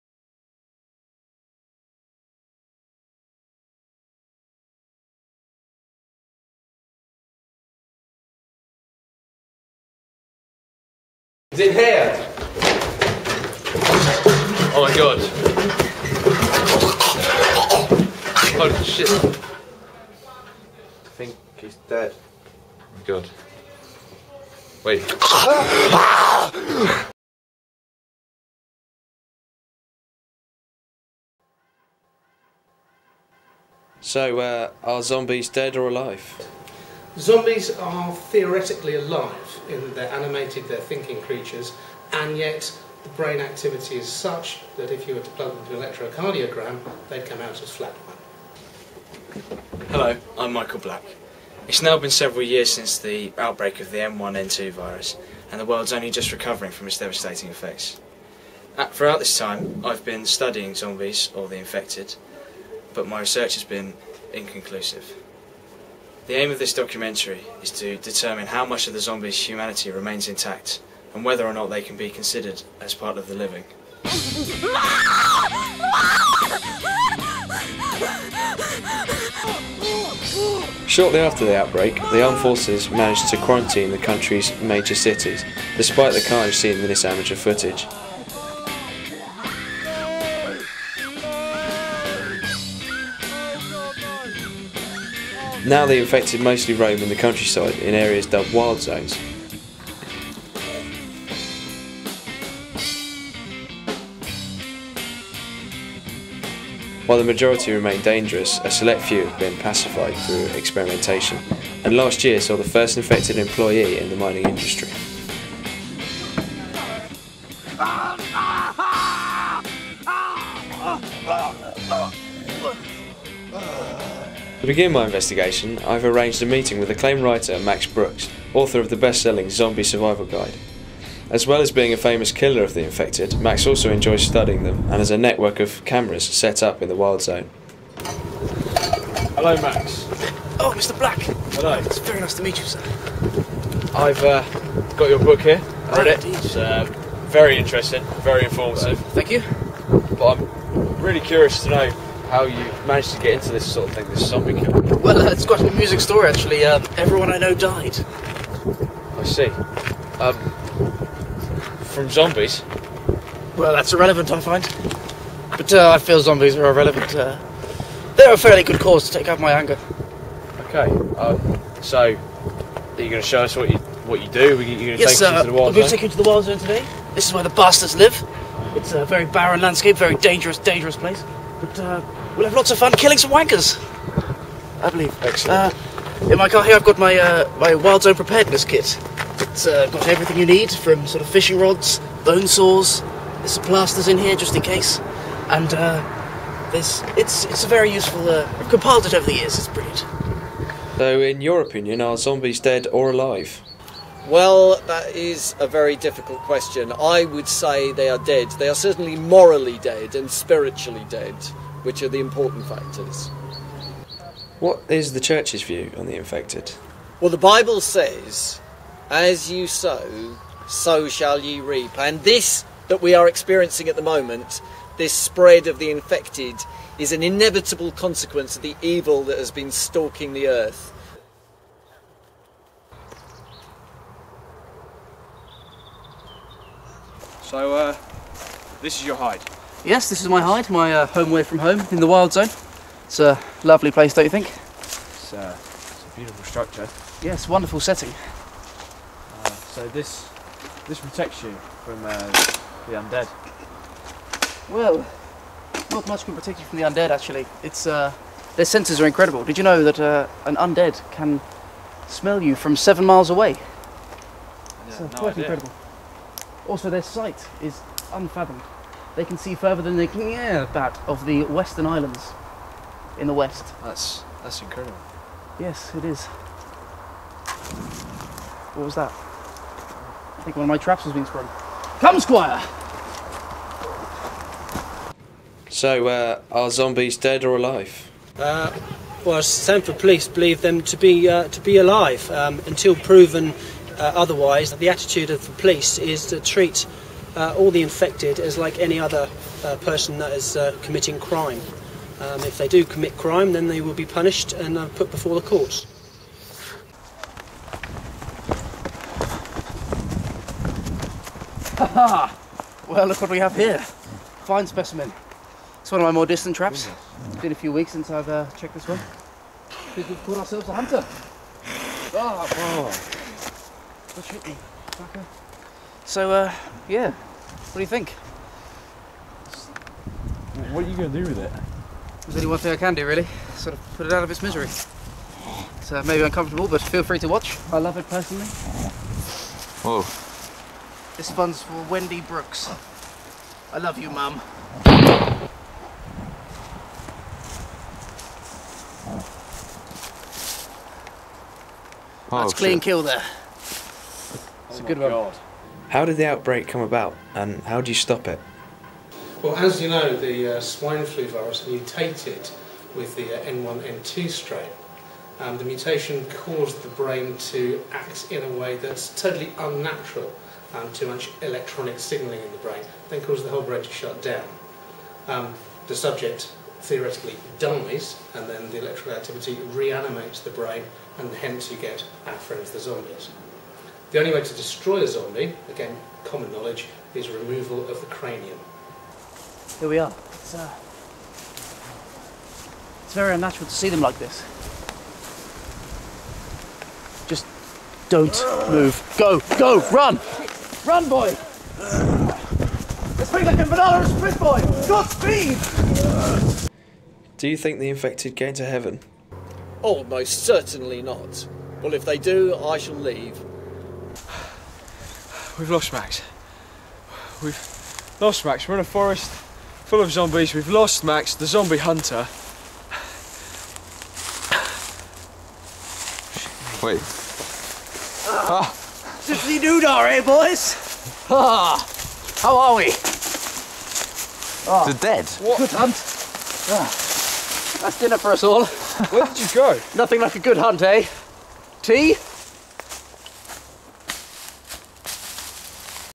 One Flew Over The Zombie's Nest - The Michael Black Story
A mockumentary made as part of our A level coursework

Part one of the critically acclaimed documentary, detailing Michael Black's investigation into the recent "zombie" virus outbreak.